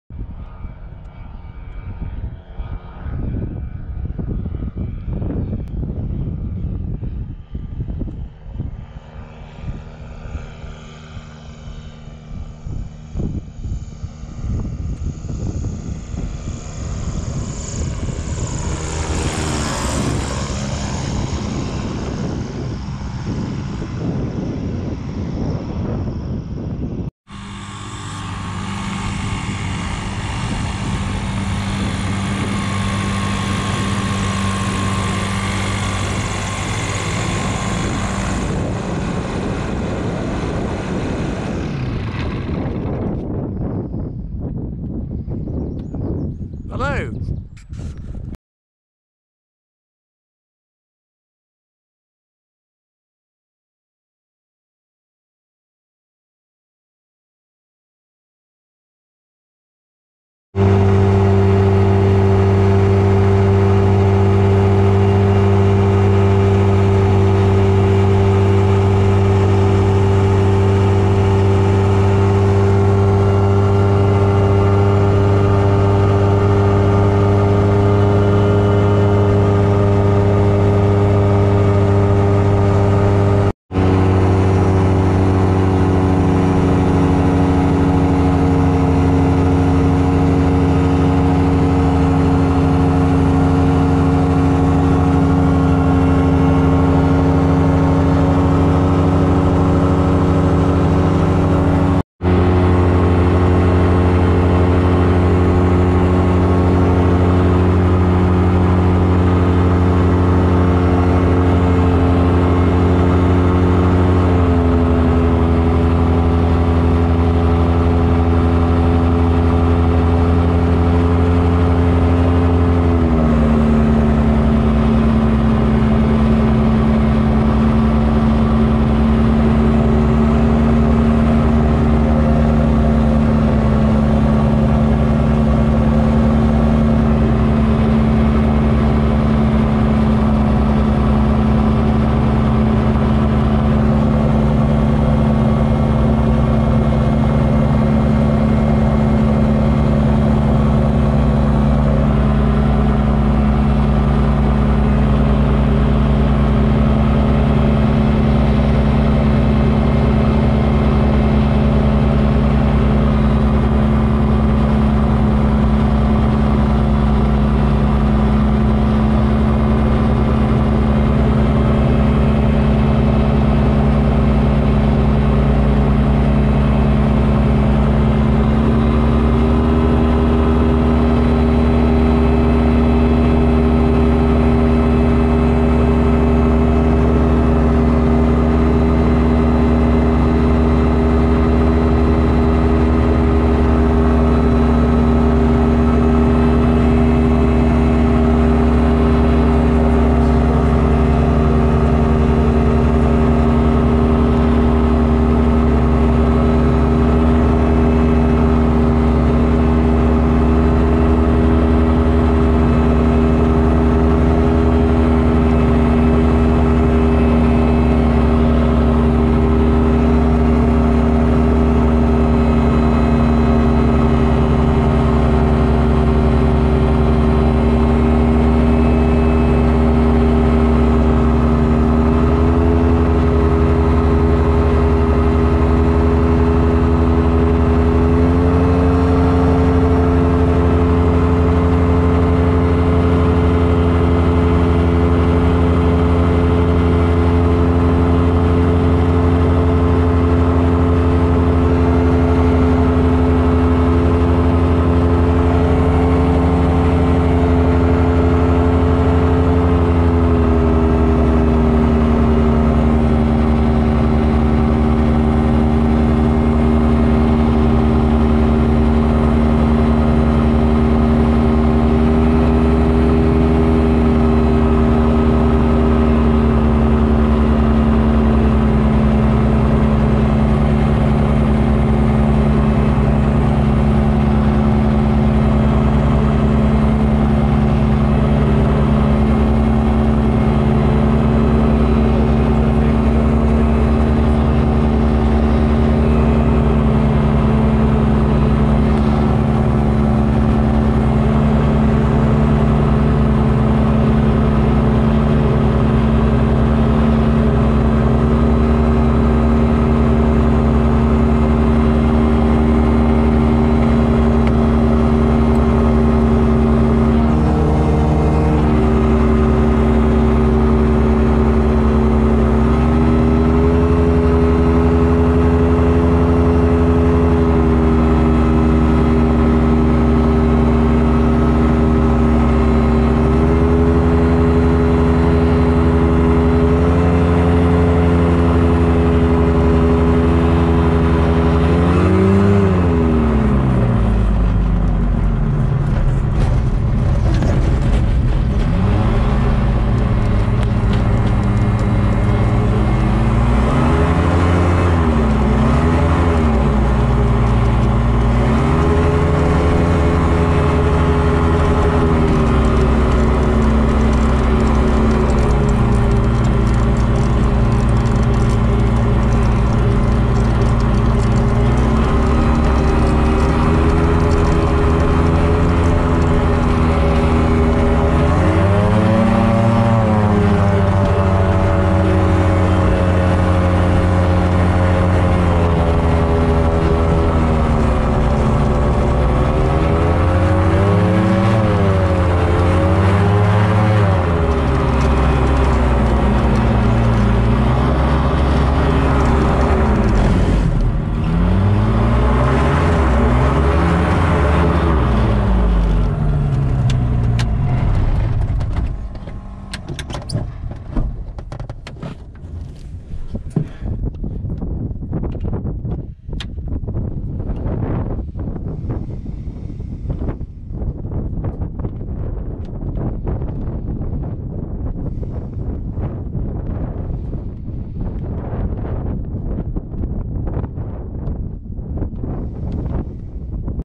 Newton Peveril to Bolt Head
The start of a tour of Devon with my good friend and fellow nutter Tom Dawson. Newton Peveril to Bolt Head nr Salcombe, where there is a Cold War nuclear bunker for regional government.